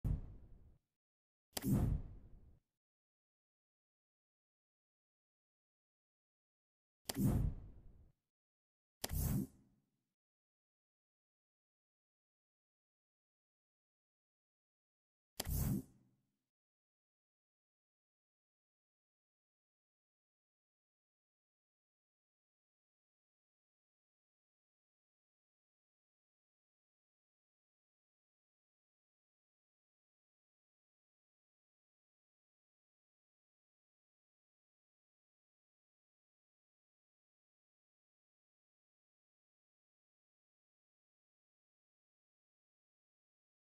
Unreal Engine 5 - Shadow Clone enemies and Axe hand weapon (+40damage)
equiping the axe changes damage from 5 to 40.
shadow clone enemies will do one attack and disappear. however, they attacks for a few times instead. I have no solution for that.